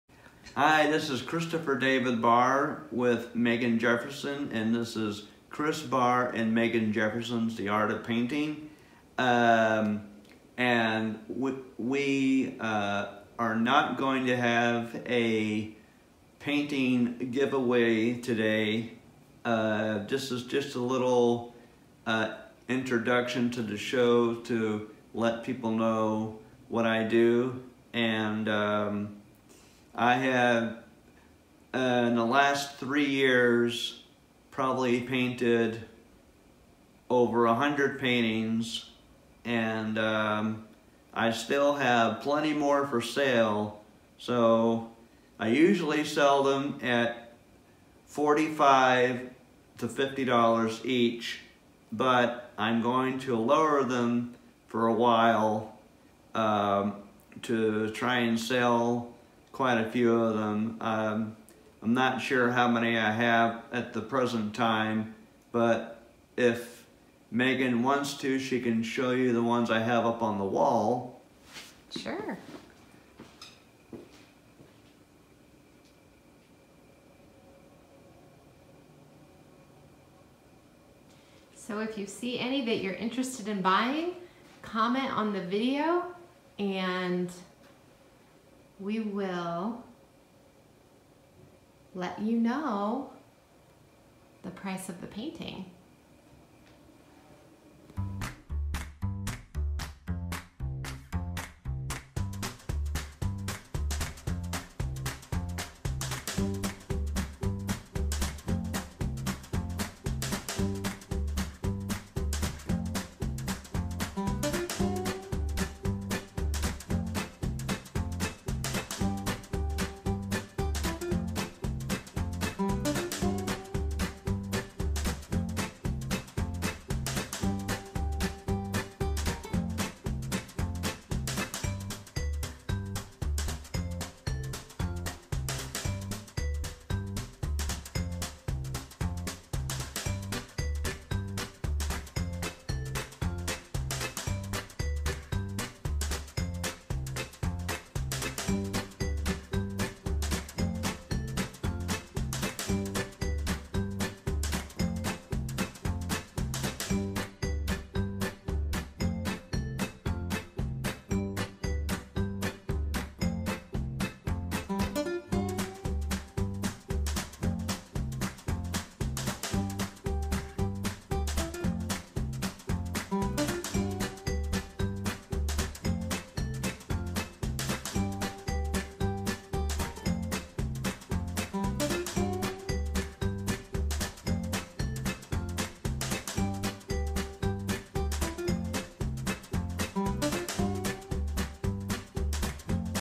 Chris Barr and Megan Jefferson's The Art of Painting, Episode 33 (4K)
Watch me create a painting. Music cred: Ben Sound